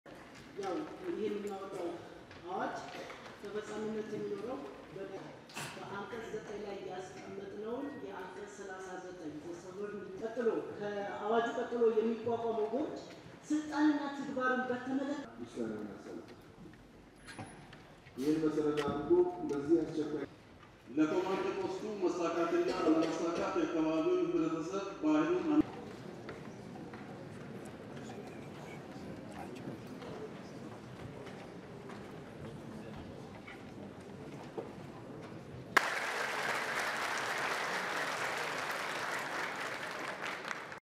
Ethiopia parliament endorses state of emergency
Ethiopia's parliament approves a nationwide state of emergency despite unusual opposition from lawmakers, in the latest sign of fractures within the all-powerful ruling coalition. IMAGES